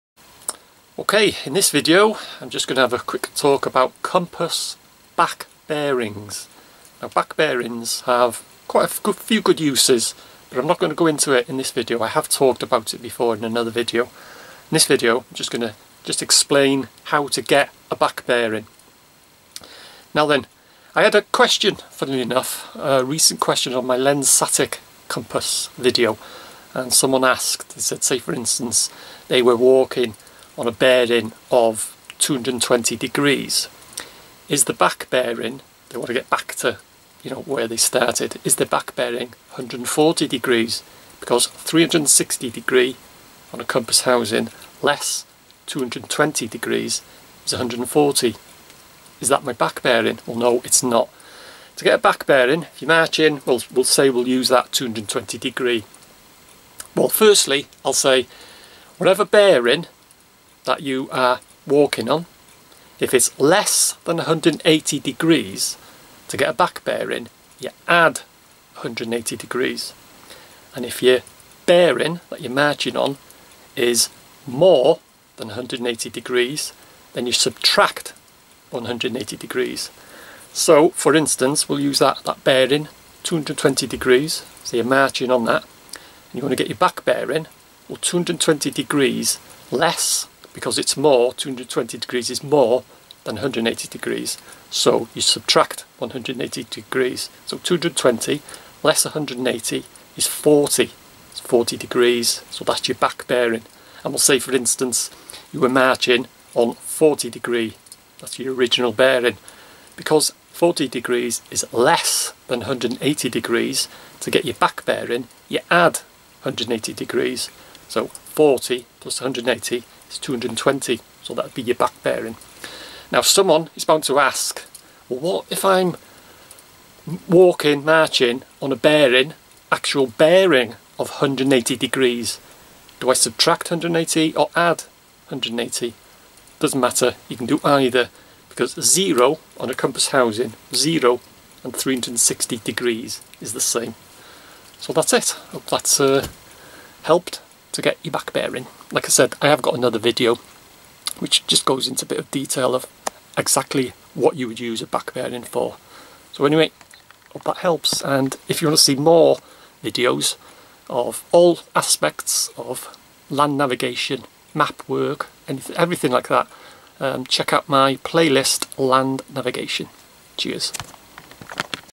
Compass back bearings
How to find a back bearing when using a Compass ... See my playlist LAND NAVIGATION for more instructional videos